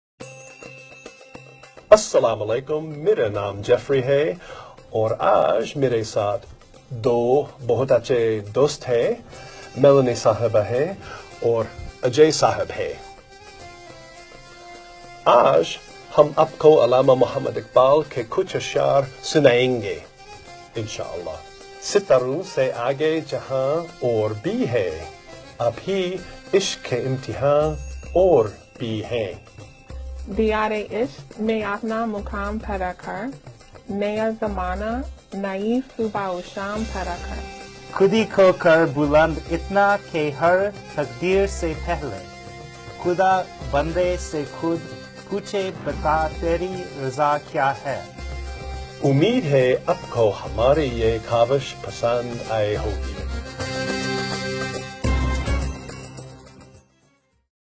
US Ambassadors Pay rich tributes to Allama Iqbal.
09/11/2017.